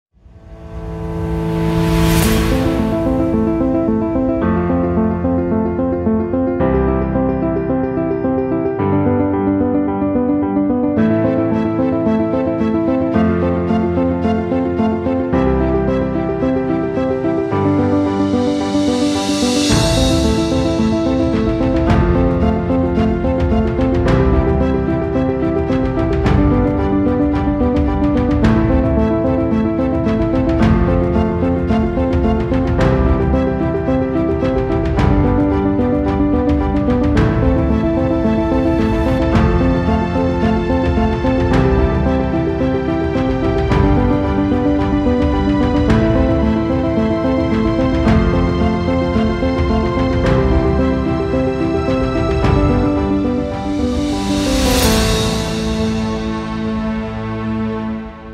Time lapse of the construction of a small biogas plant in France
The biogas plant was installed in Steenbecque in February 2023. The video shows the construction of a small digester with a diameter of 13.5 meters and a gross volume of 508 cubic meters.